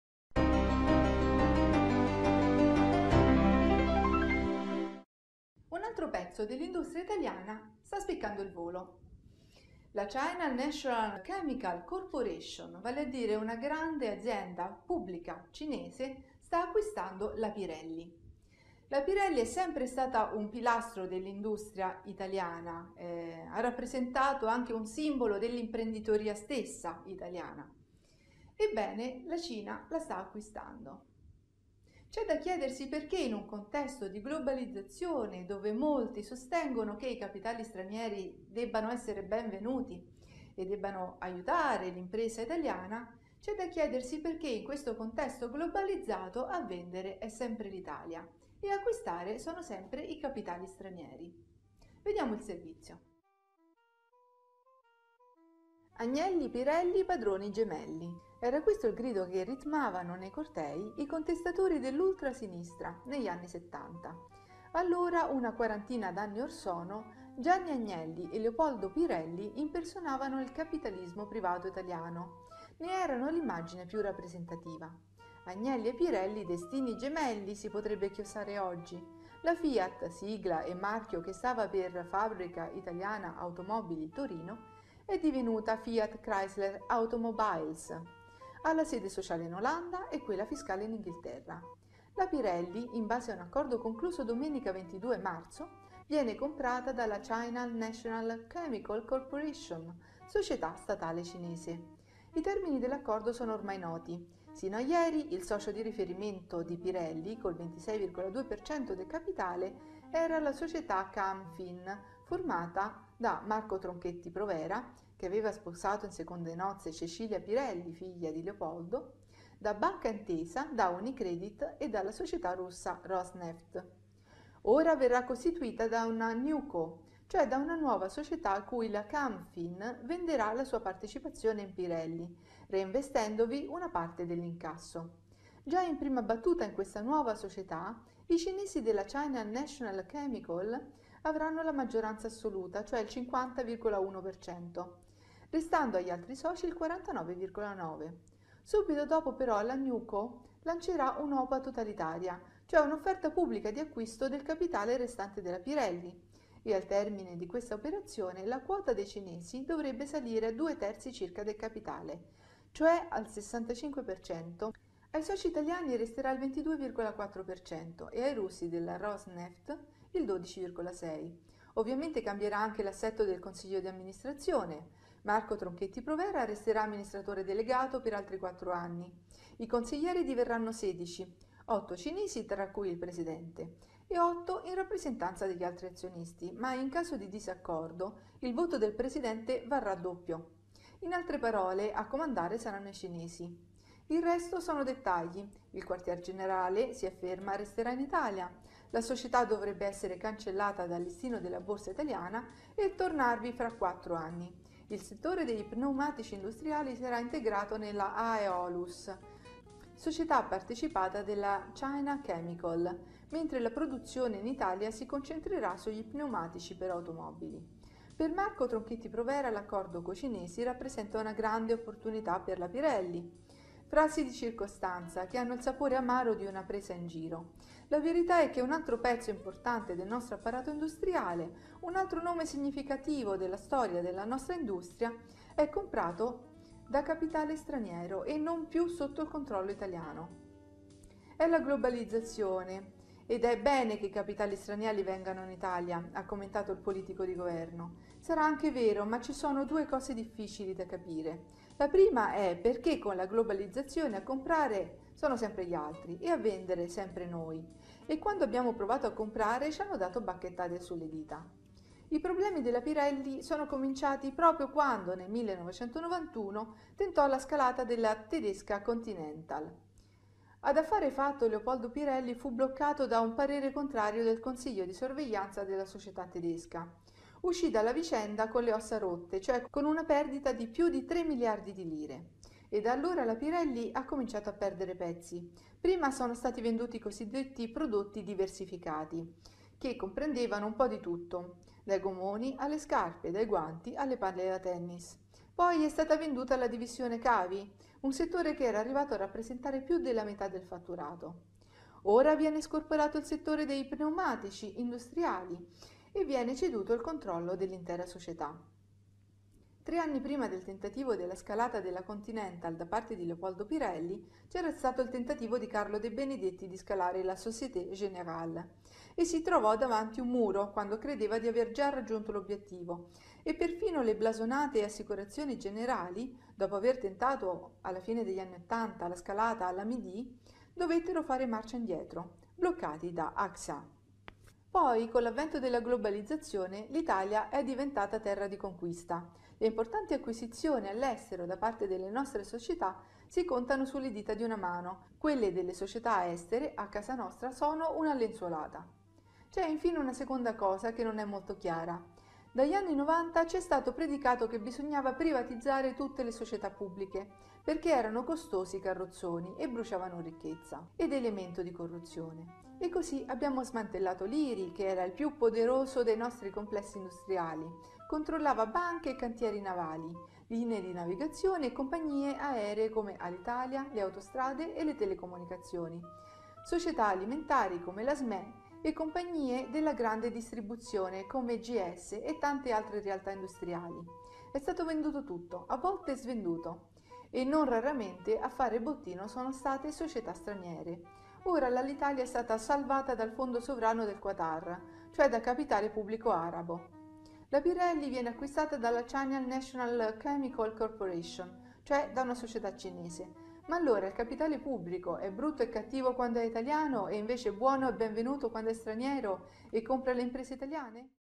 La Pirelli cinese: un altro pezzo dell'industria italiana in mani straniere
Un'altro pezzo dell'industria italiana spicca il volo. Si tratta della Pirelli, uno dei simboli del miracolo economico italiano e della Milano da bere.